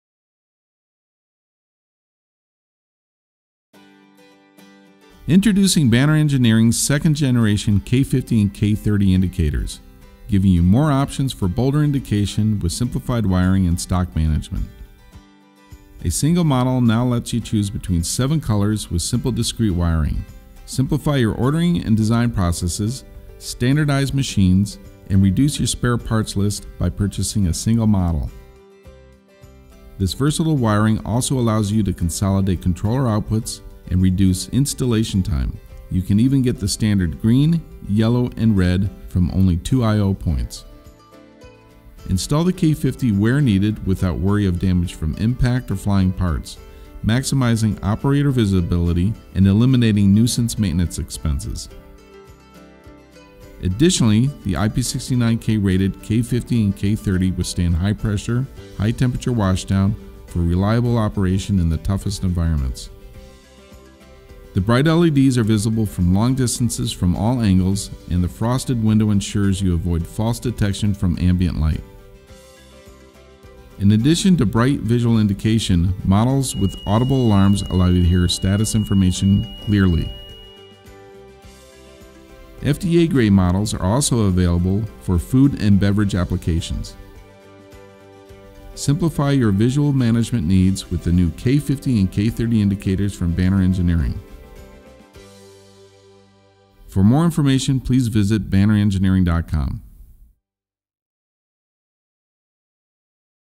Banner K50L2 and K30L2 Indicators: Bolder Indication for Everyone, Anywhere
Banner Engineering's second generation K50 and K30 domed indicators give you more options for bolder indication with simplified wiring and stock management. Choose between seven colors with a single, rugged model.